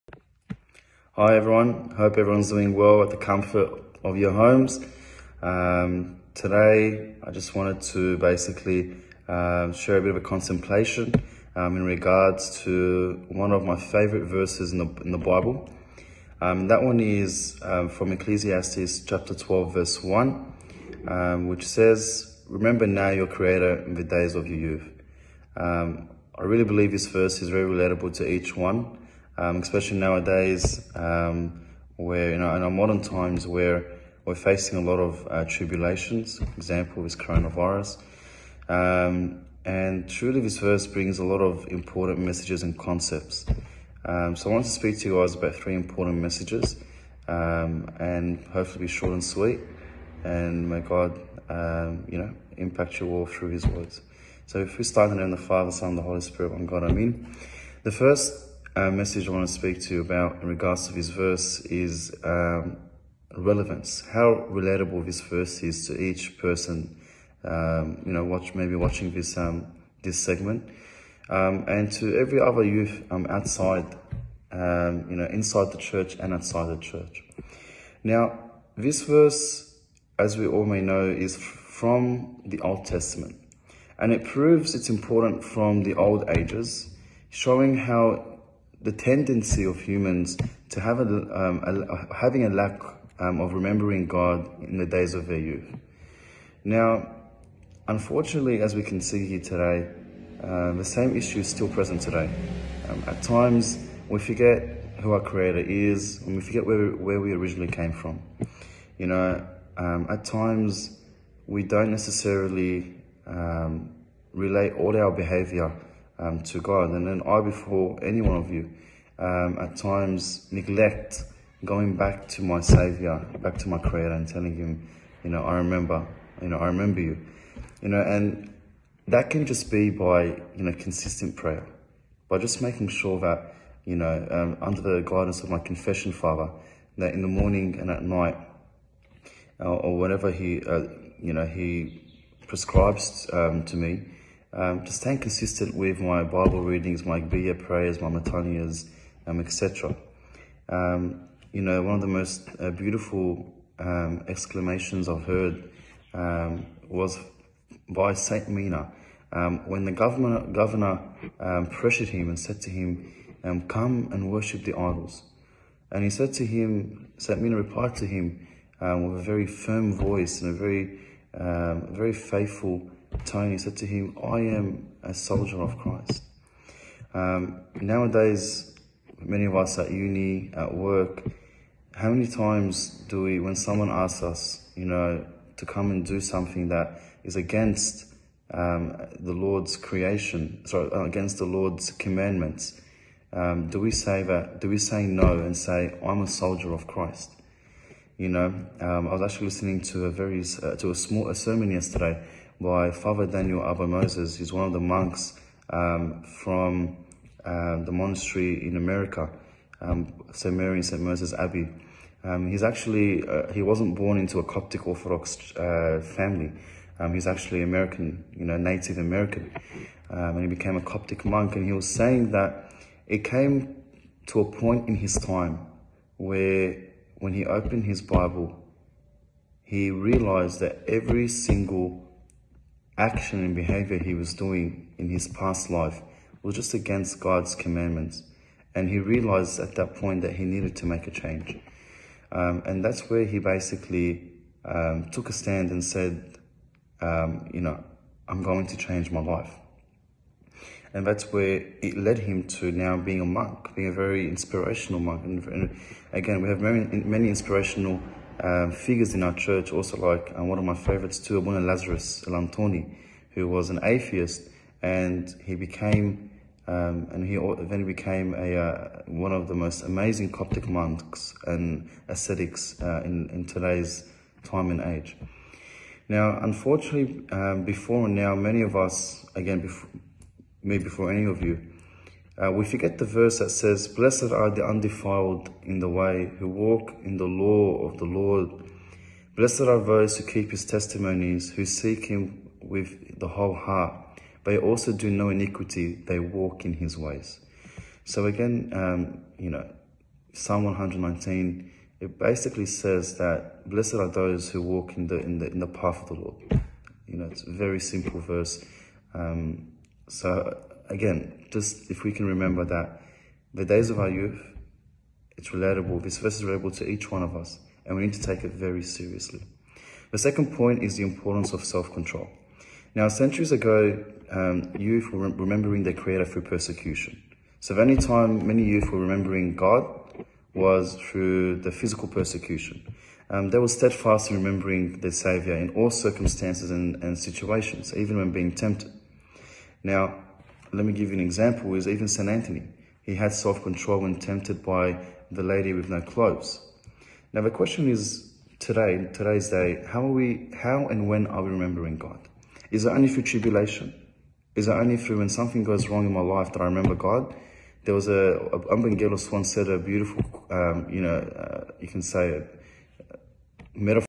Youth Segment 1 - Abanoub Mikhail
| YOUTH SEGMENT |

SAYG!

Check out this beautiful segment by our very own Abanoub Mikhail where he reflects on the famous verse "Remember now your Creator in the days of your youth" Ecclesiastes 12:1!